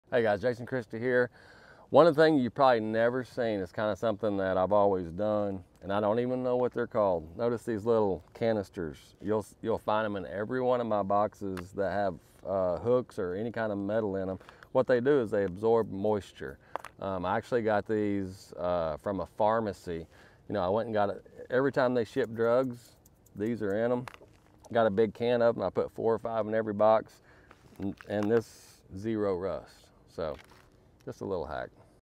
Anti-RUST Tackle Hack with Jason Christie🧠
Try adding moisture absorbing pods to your tackle boxes in order to avoid rust. 👍